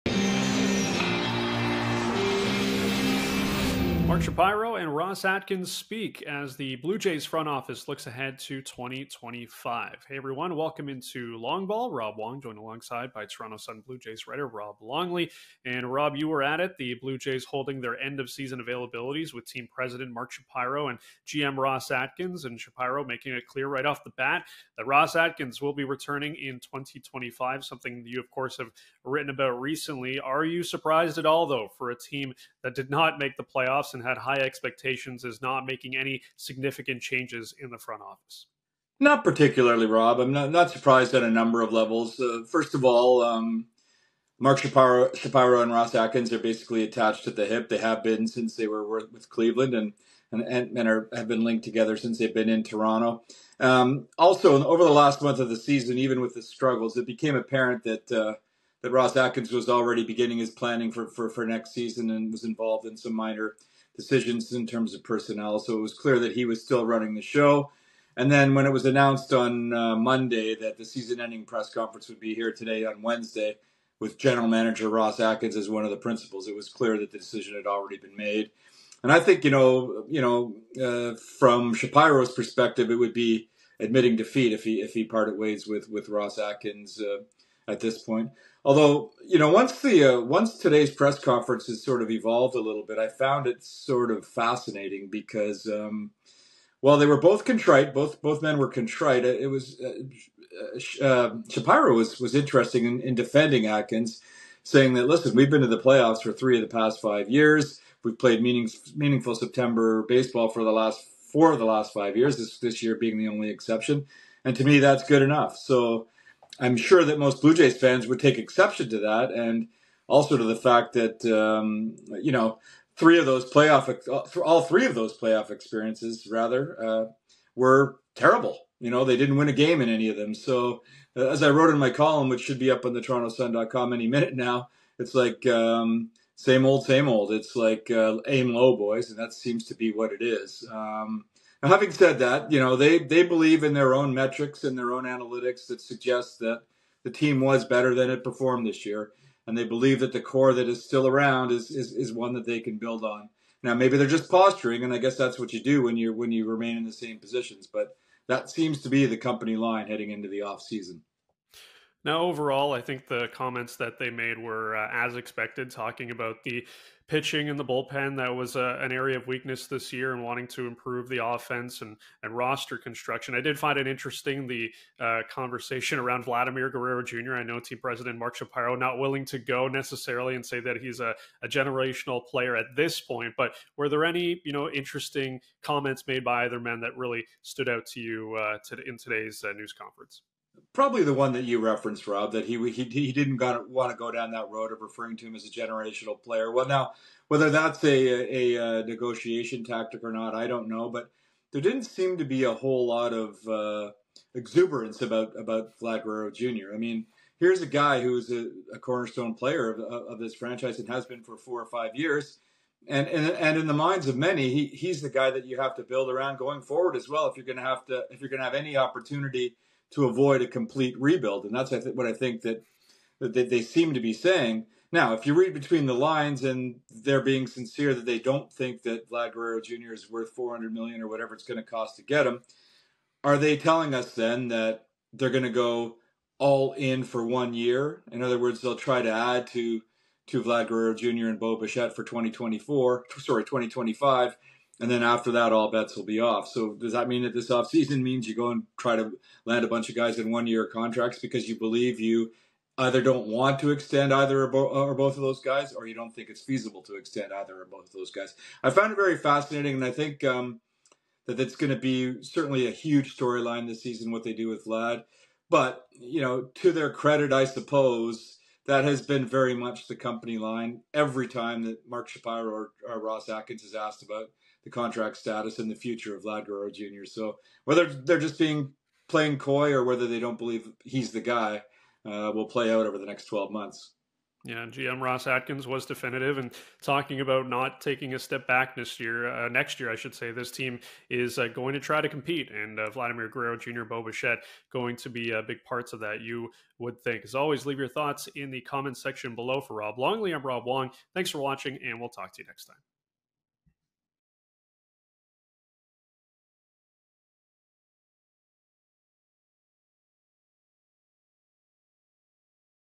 Long Ball: Ross Atkins remains Jays GM for 2025
WATCH BELOW as Postmedia’s Rob Wong chats with Toronto Sun Blue Jays writer Rob Longley about Ross Atkins returning as Blue Jays general manager, the biggest takeaways from the end-of-season availabilities from Atkins and team president and CEO Mark Shapiro and the team's plans for the offseason.